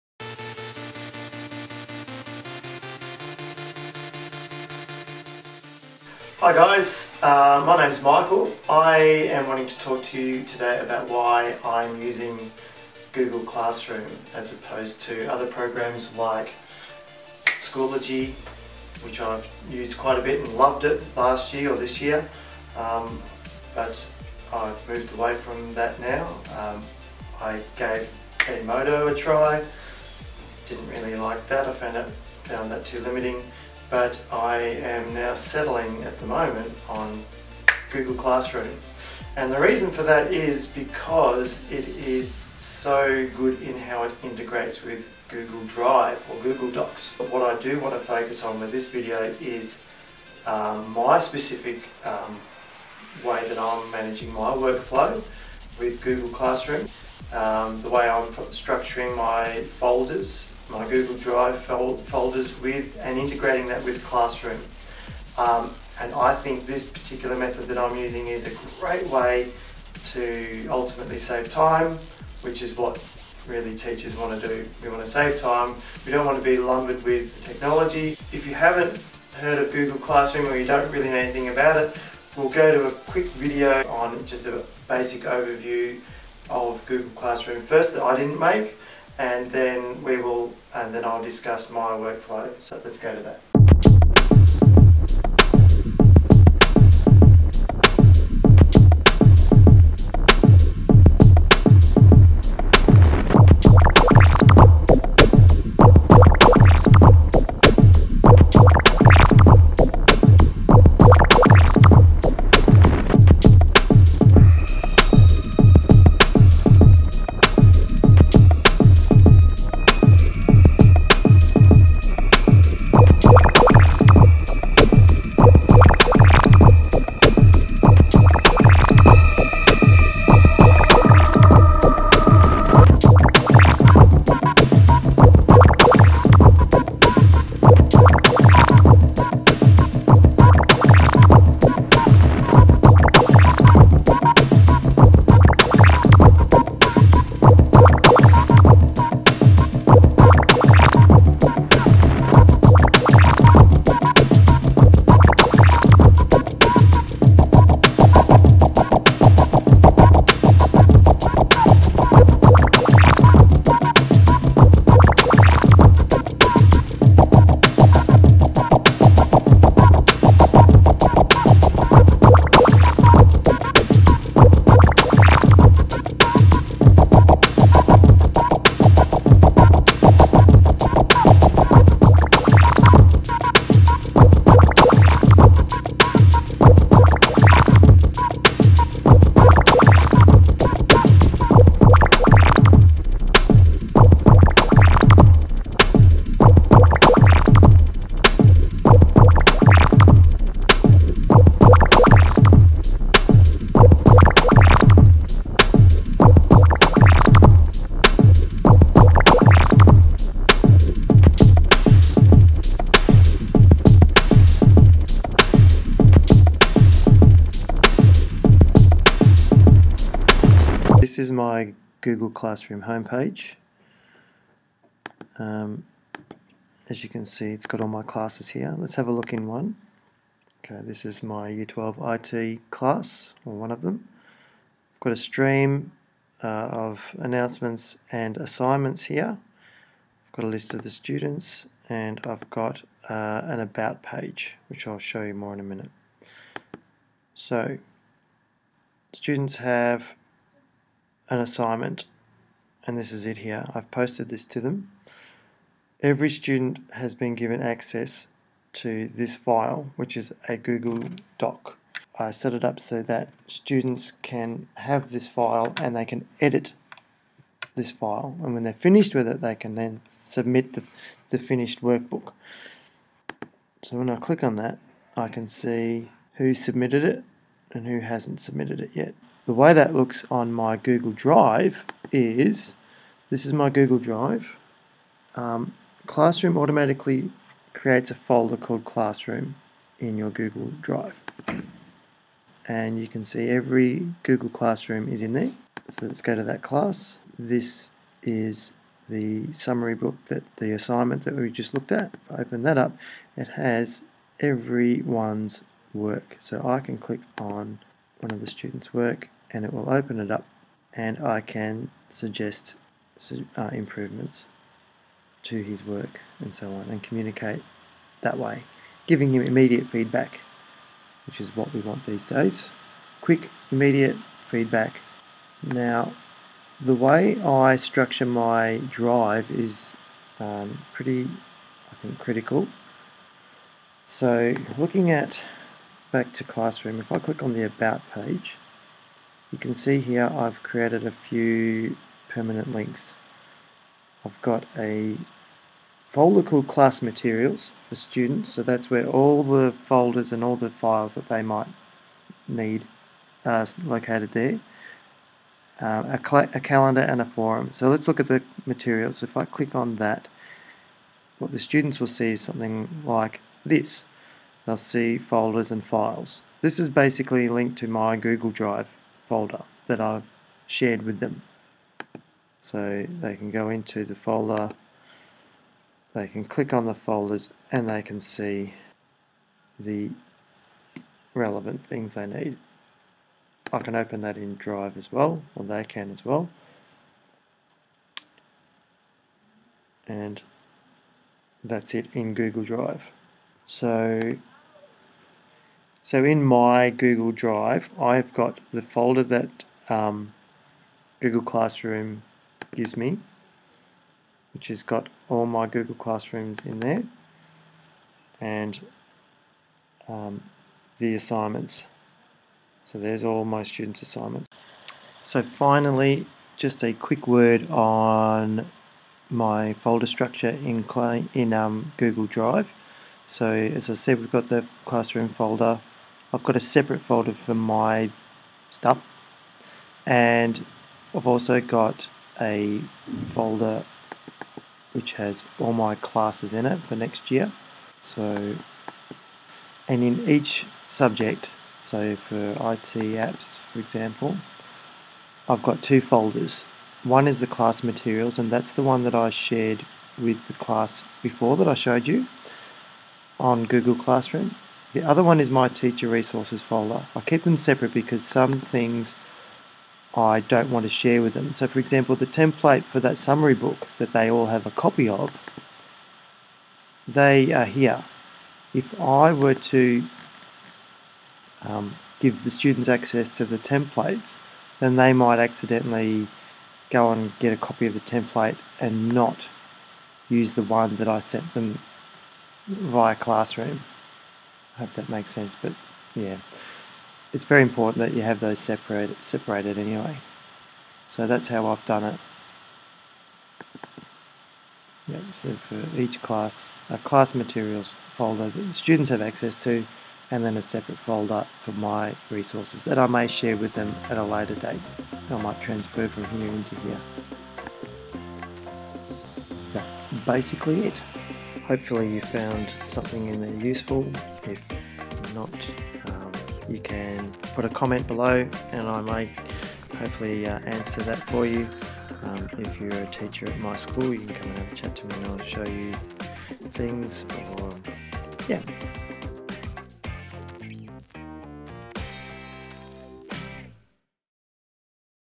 My Google Classroom Workflow Solution
This video focuses on my workflow solution using Google Classroom's strong integration with Google Drive.